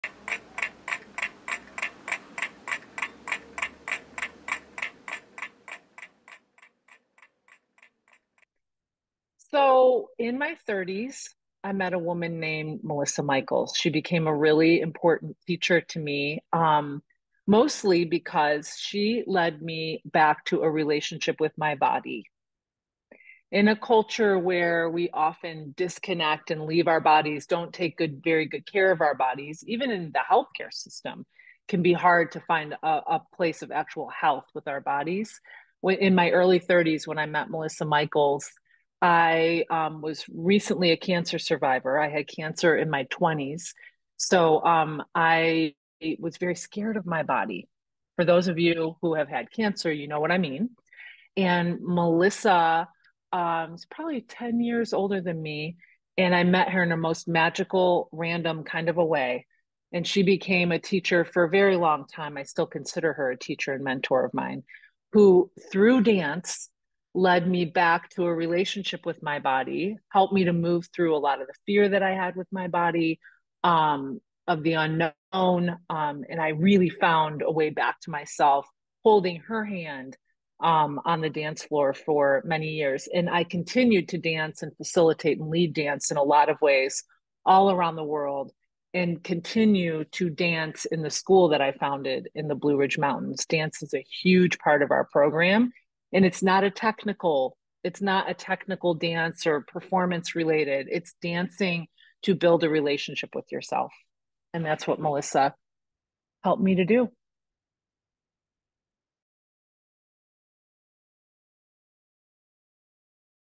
Jenny Finn - Teachers Make a Difference - Melissa Michaels
Jenny Finn has designed structures that foster vitality in people, communities, and organizations for nearly 30 years. She holds a Ph.D. in Sustainability Education and co-founded Springhouse, a school inspired by a vision to live in a world where all life thrives. Dr. Finn’s research, mentoring, and teaching invites people to deepen the relationship they have with themselves in order to serve the world with greater clarity and compassion. Her work has taken many forms including non-profit leadership, trauma and hospice care, chaplaincy, clinical private practice, community building through the expressive arts, and education. With a dedicated and inspiring community, Dr. Finn articulated Sourced Design – five principles that take care of life in a person, community, and place. These principles are practiced at Springhouse and shared globally with those who share the vision of a thriving world.